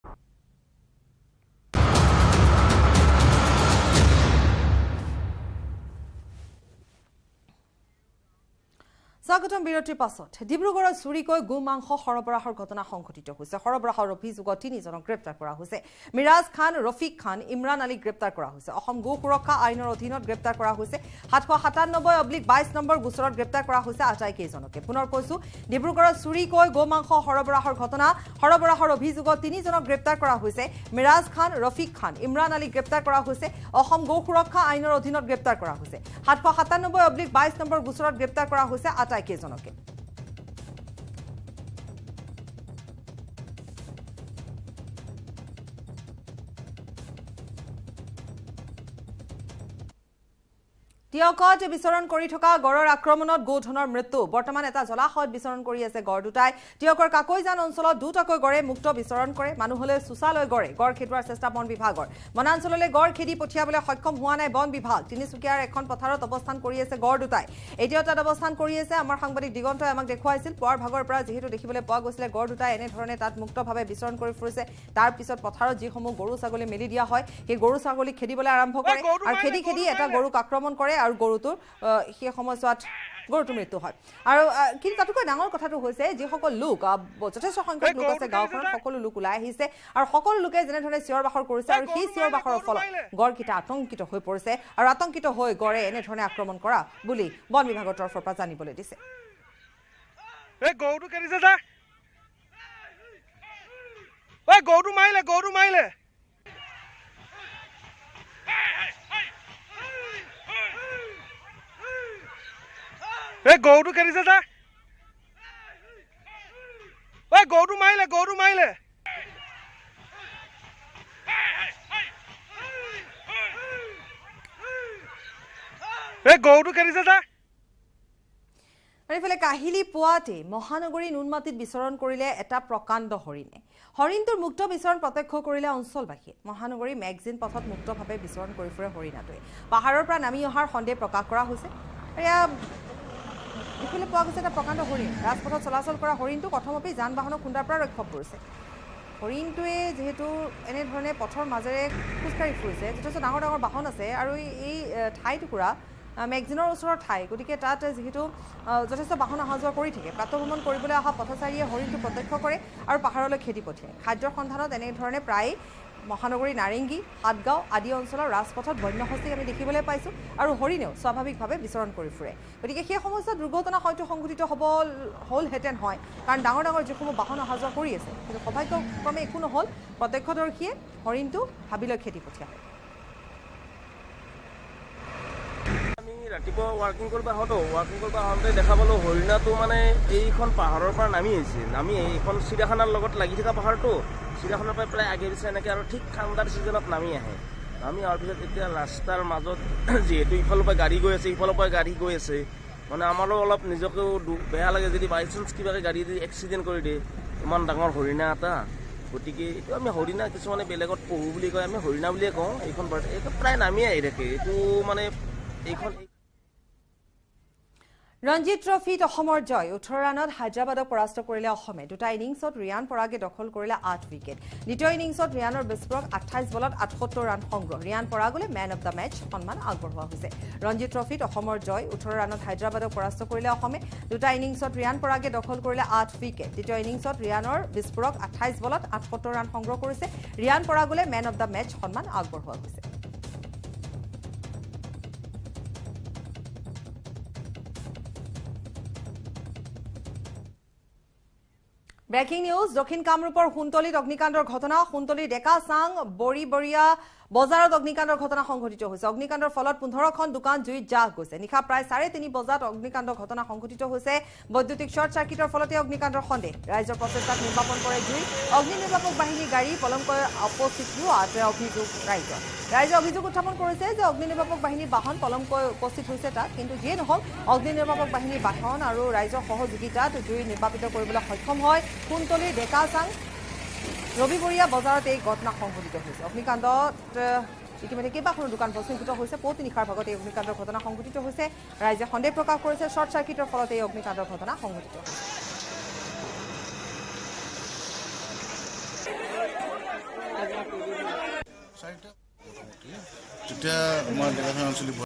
Cow meat smuggle news : গো মাংস সৰবৰাহৰ অভিযোগত ৩ জনক গ্ৰেপ্তাৰ | 3 arrested for smuggling cow meat
Cow meat smuggle news | Dibrugarh ত চুৰিকৈ গো মাংস সৰবৰাহ । সৰবৰাহৰ অভিযোগত ৩ জনক গ্ৰেপ্তাৰ